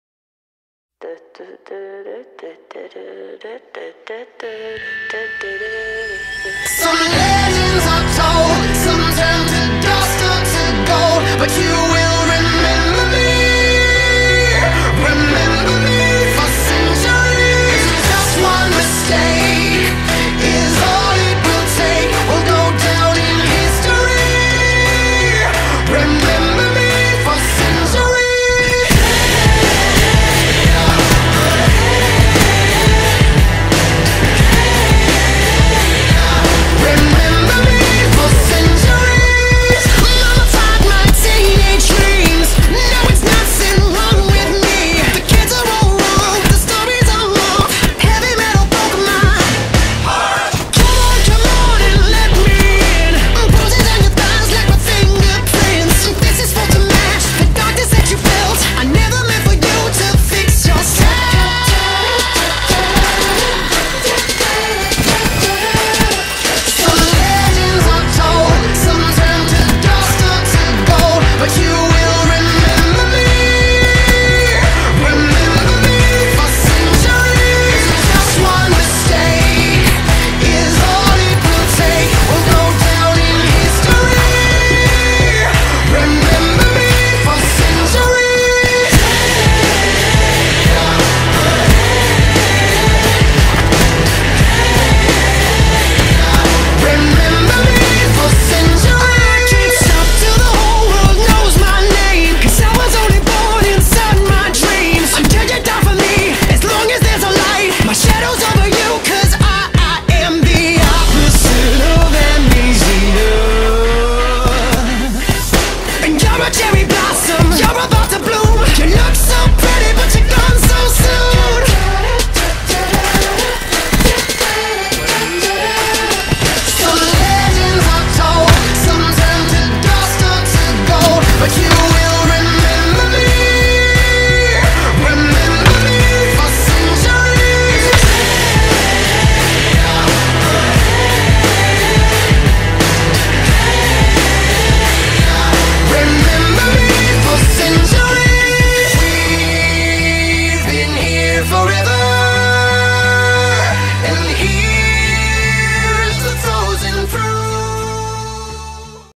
BLE3EDING's 250 SUBS MONTAGE ! Thank you guys !
Thank you guys for 250 subs !! here is a short triples montage for you awesome ppl !
If you enjoyed any of my original music i've added to the video, Listen or Download away!